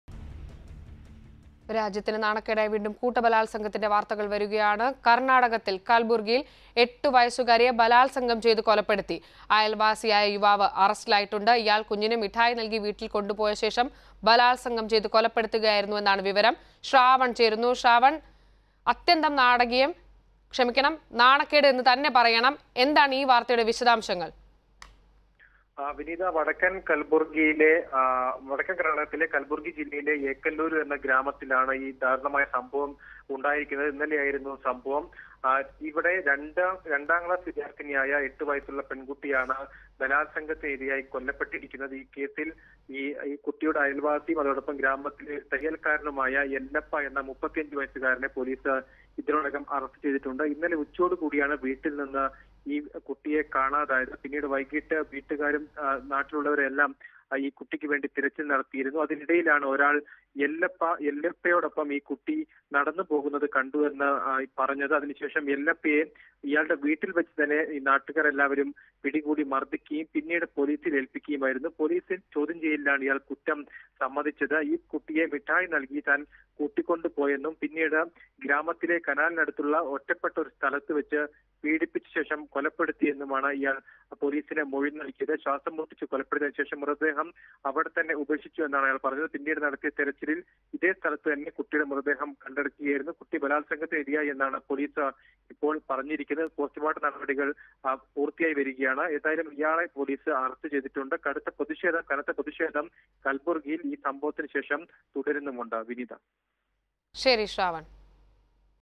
കര്‍ണാടകയില്‍ എട്ട് വയസുകാരിയെ ബലാത്സംഗം ചെയ്ത് കൊന്നു: അയല്‍വാസി അറസ്റ്റില്‍
8 year old raped in Karnataka ; Neighbor arrested